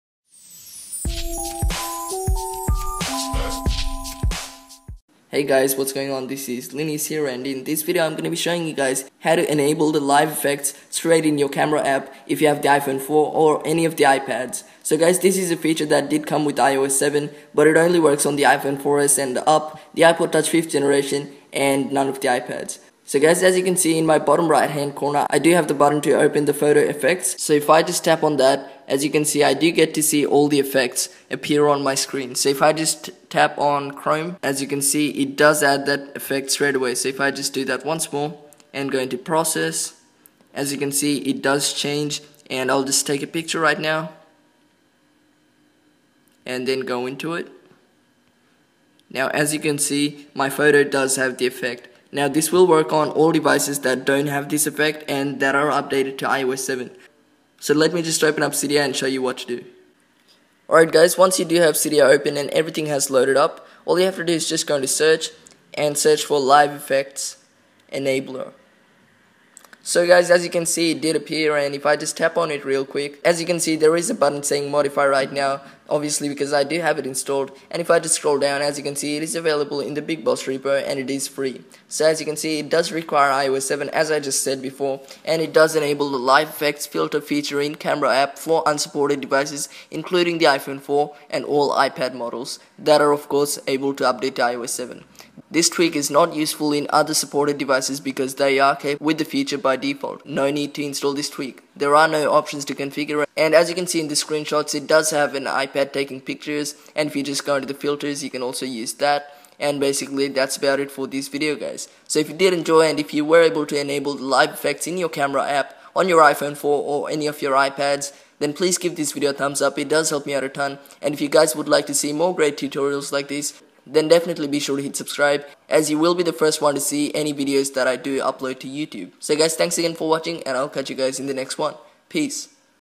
iOS 7 Tutorials - How To Enable Live Photo Effects On iPhone 4 and All iPads
In this iOS 7 Tutorial, I will be showing you how to get the Live Photo Effects on the iPhone 4 and All iPads including iPad Air/4/3/2 iPad Mini/iPad Mini With Retina Display.